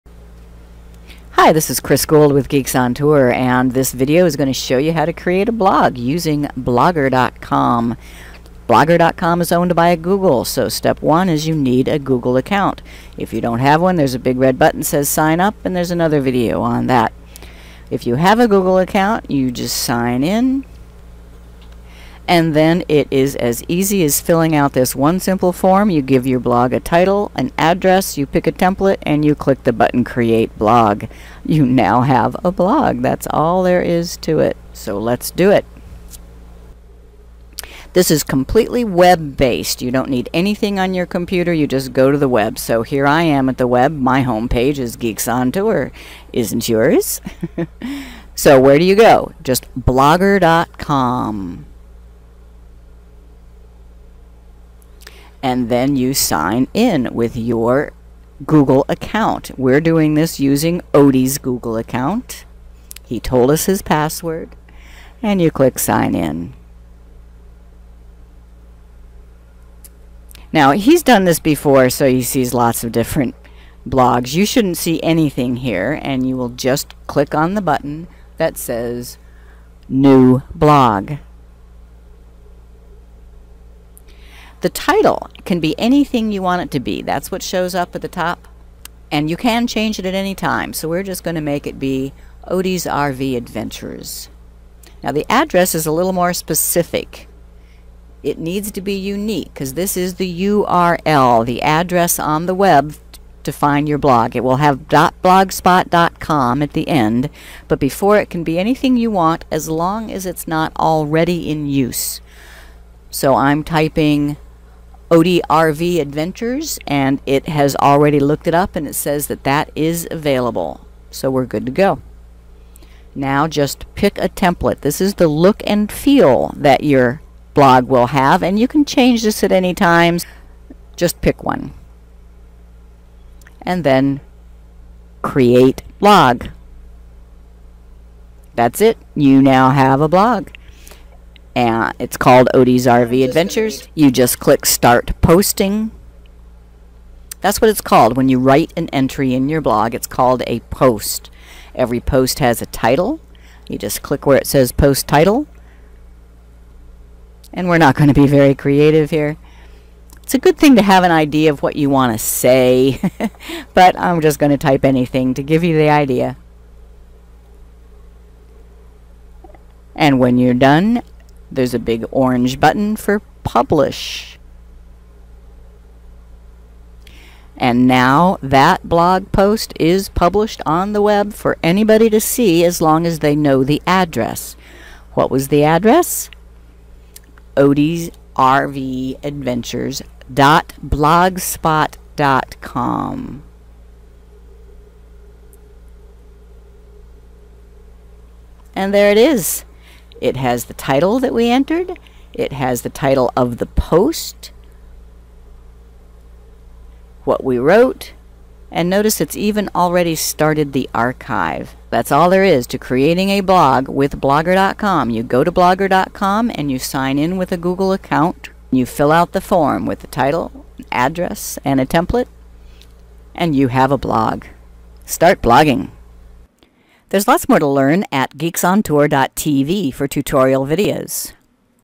Create a Free Blog in 3 Steps using Blogger.com
A Tutorial Video by Geeks on Tour. Blogger.com is a free website by Google where you can make a Blog or website. This short video will show you how. Lots of Travelers use this for their travel logs. See lots more at GeeksOnTour.com Tutorial Videos